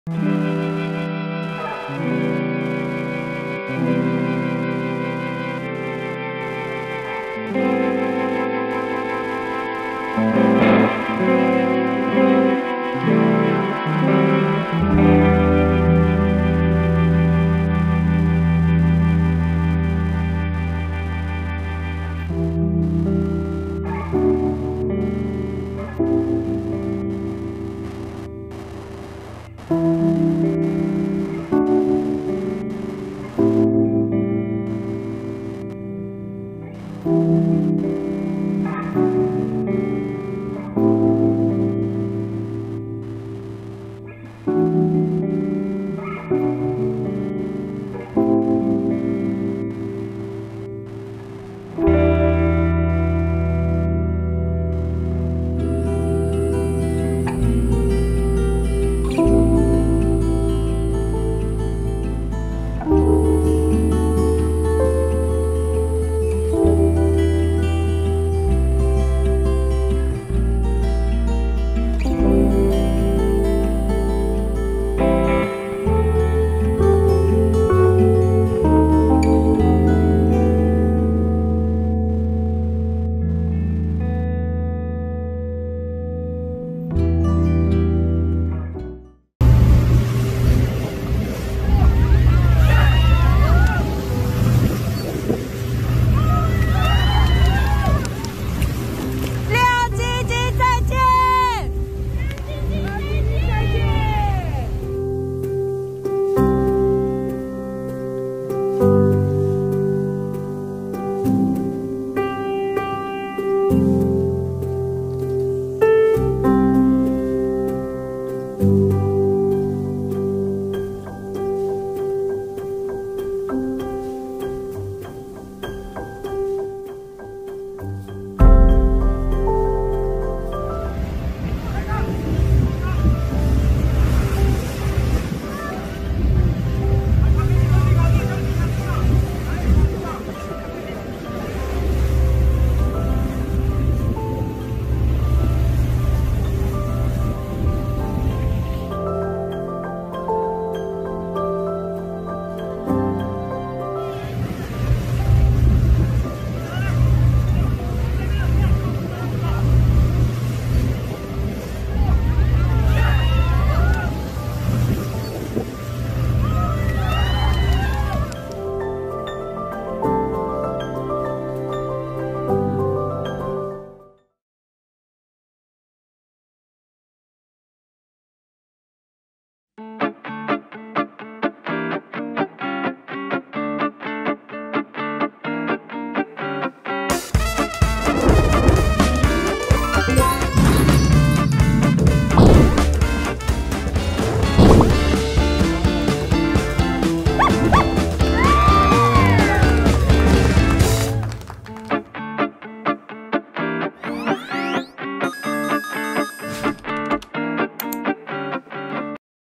澳洲海岸驚現鯨屍 230頭領航鯨離奇擱淺｜鯨魚｜野生動物｜海洋生物
☑影片授權：Various、AuBC／AP